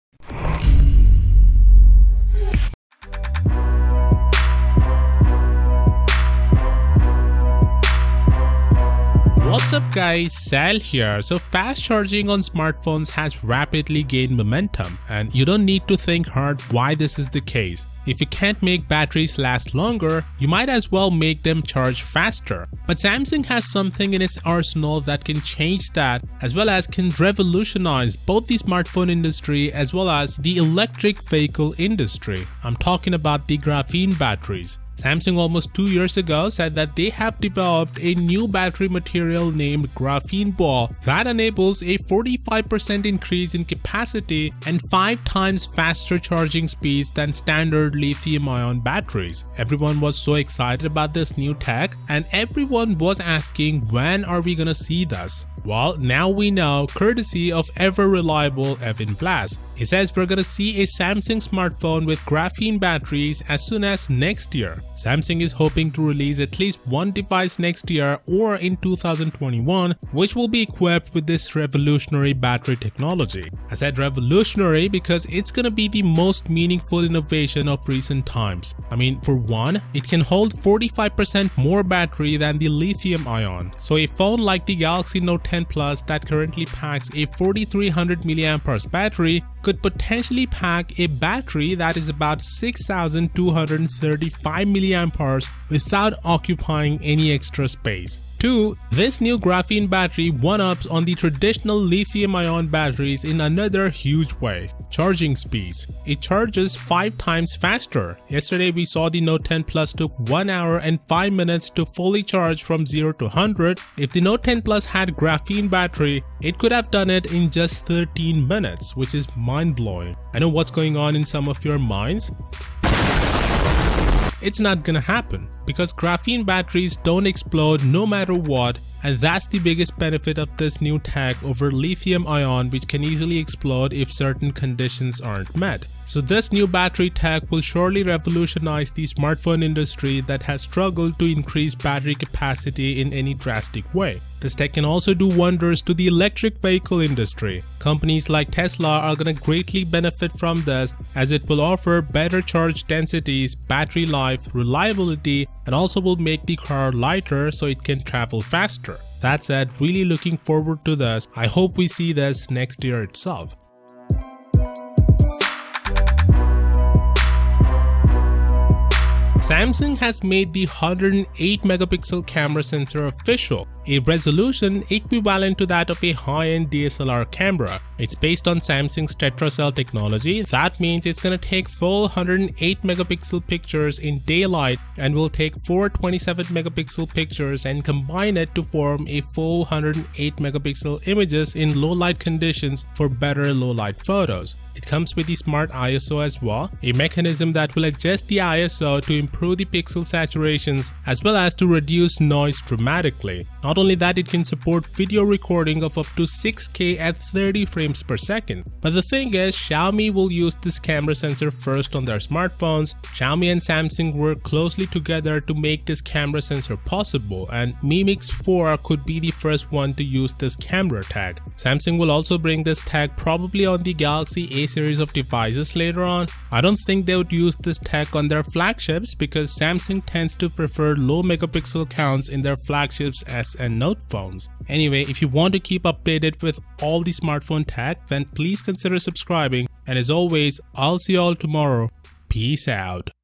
Samsung Graphene Battery Smartphone Is NEAR
Samsung will launch a smartphone with a graphene battery as soon as next year. If the Samsung Galaxy Note 10 Plus had graphene battery it could have had 6235mAh battery in the same size. Also, the Galaxy Note 10+ could be able to charge the phone in just under 13 minutes if it had the graphene battery, of course.